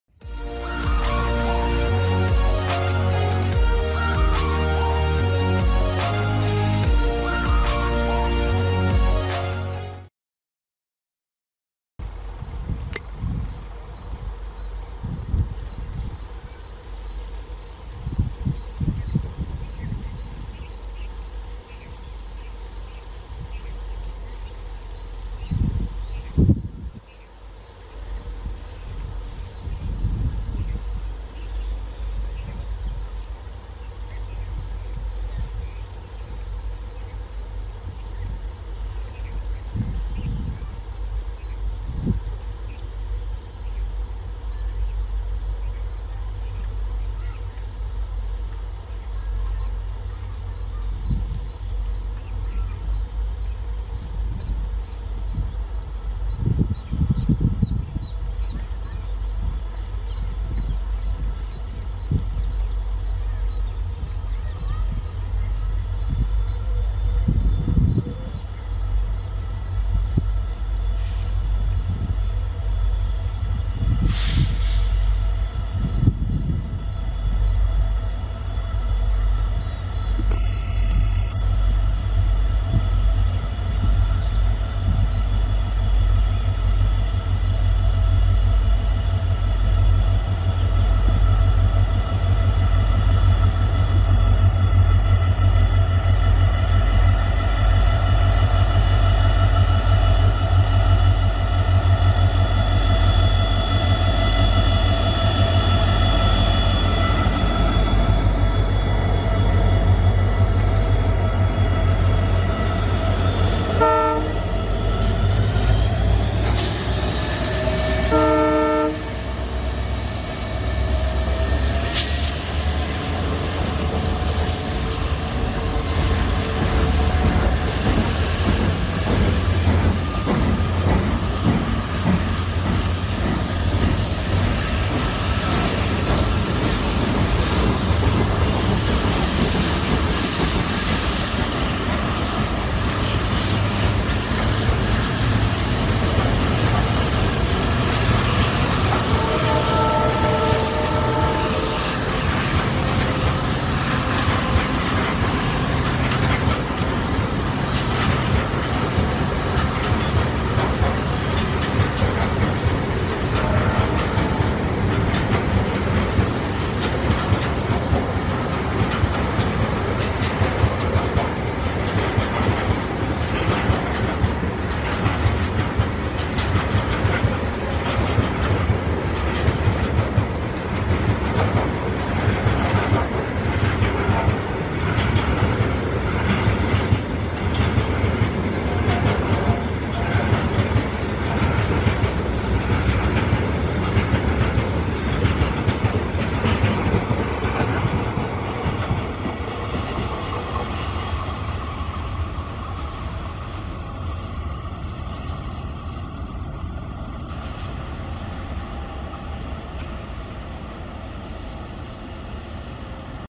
TWIN WDG 4 with BCNA wagon climbing Gradient and beautiful EMD acceleration
amazing EMD acceleration  after struggling to climb gradient with BCNA wagons
loco- SBI -WDG-4 12956 ,GY- WDG-4 40450
location :Arsikere
loco-KJM WDP-4D  40450